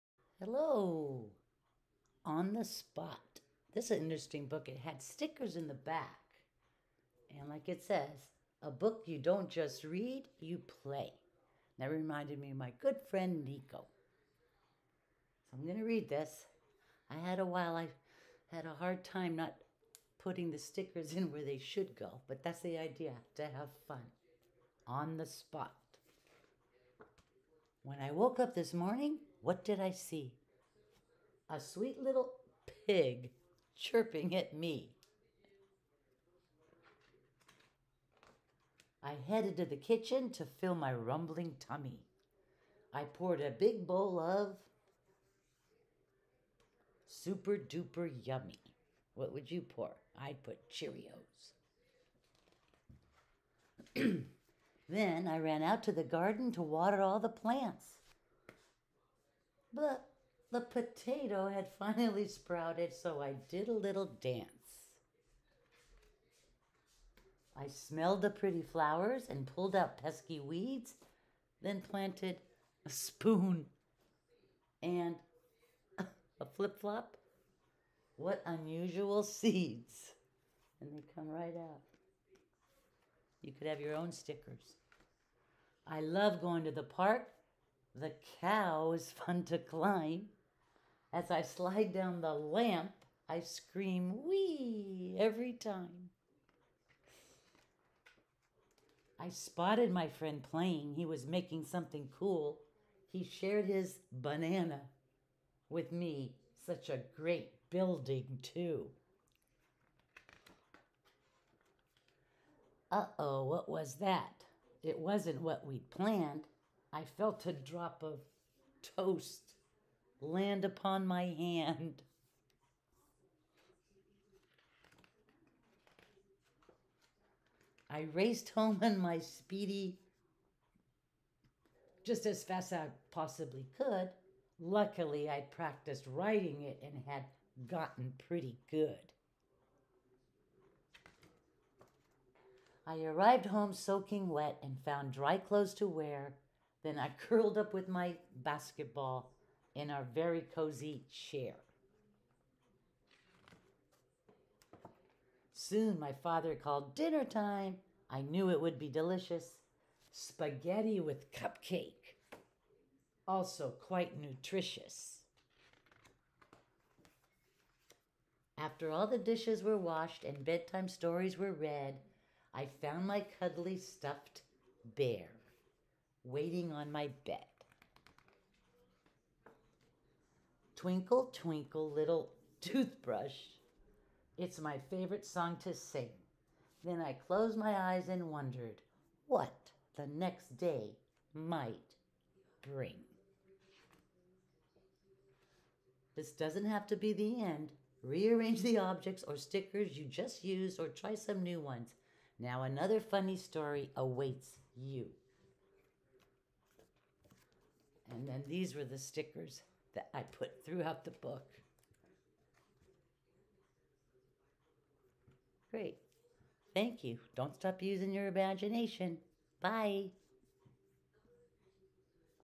Mrs. Stathis reads "On The Spot"
Retired teacher Mrs. Stathis reads to kids at home and young at heart.